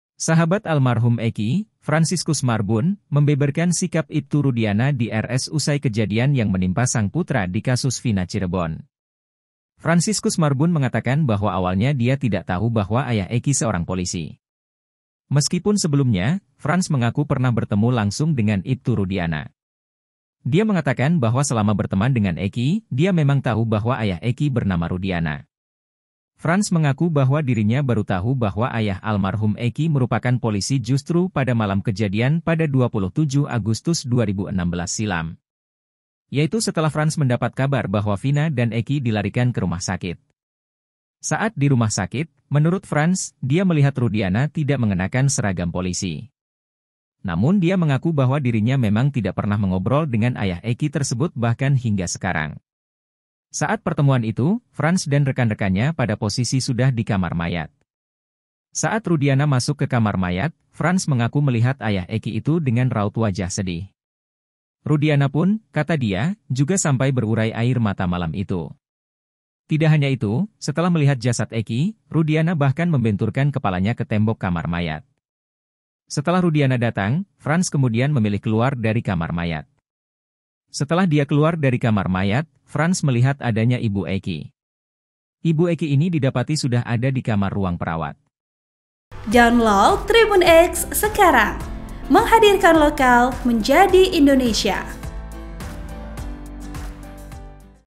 Sikap Iptu Rudiana di RS Pasca-Kejadian Diungkap Sahabat, Akui Baru Tahu Ayah Eky Polisi
TRIBUN-VIDEO.COM - Sahabat almarhum Eky, Fransiskus Marbun, membeberkan sikap Iptu Rudiana di RS usai kejadian yang menimpa sang putra di kasus Vina Cirebon.

Fransiskus Marbun mengatakan bahwa awalnya dia tidak tahu bahwa ayah Eky seorang Polisi.

Meskipun sebelumnya, Frans mengaku pernah bertemu langsung dengan Iptu Rudiana.(*)

Editor Video: Reka Alfa